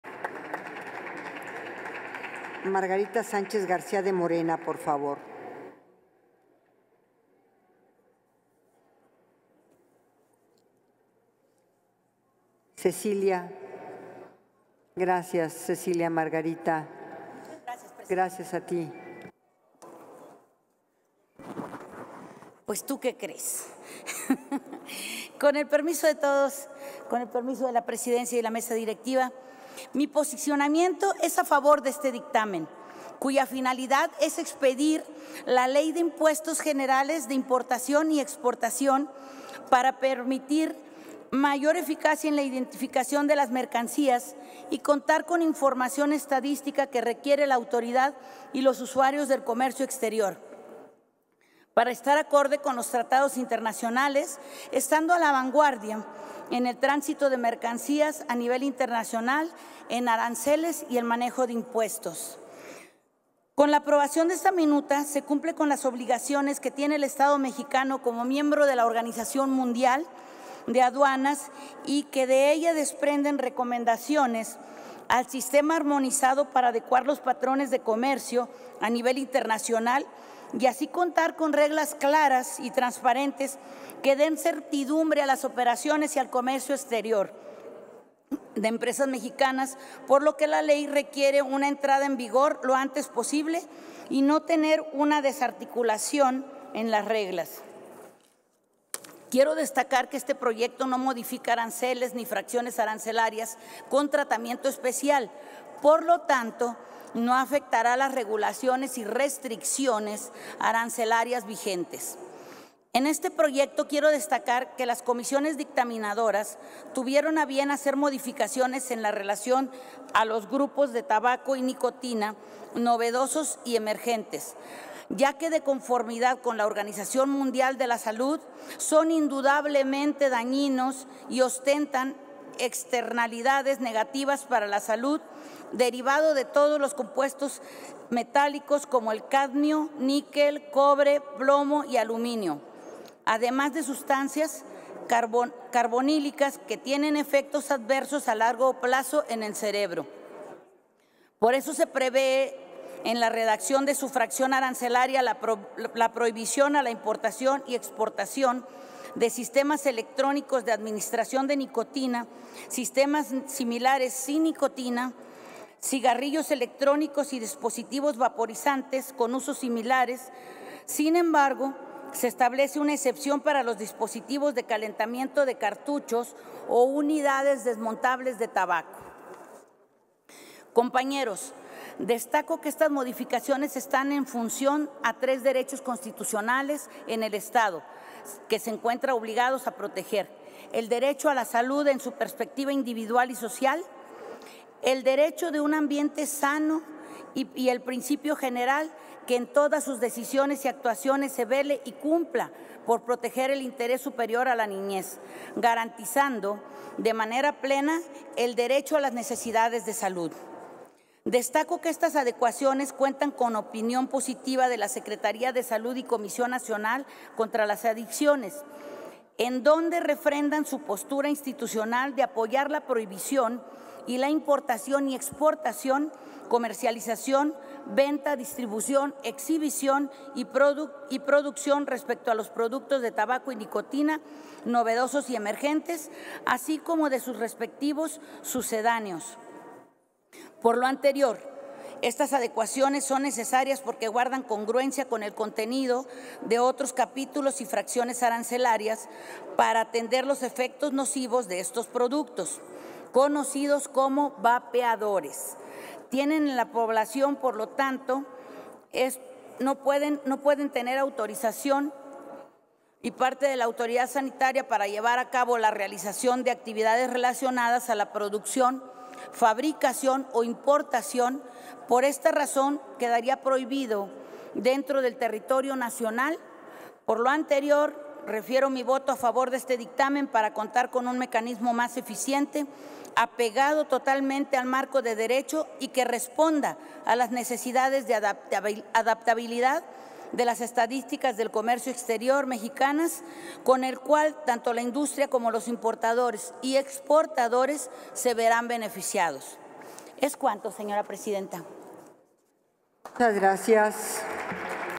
Vapeadores son indudablemente dañinos: Sen. Cecilia Sánchez (Morena)
La senadora Cecilia Sánchez, de Morena, señala que se busca proteger el derecho a la salud y velar por el interés superior de la niñez. Añade que con el presente dictamen se busca una mayor eficacia en la identificación de mercancías y contar con la información estadística que requiere la autoridad  aduanera.

Lo anterior, al referirse al dictamen por el que se expide la Ley de los Impuestos Generales de Importación y de Exportación.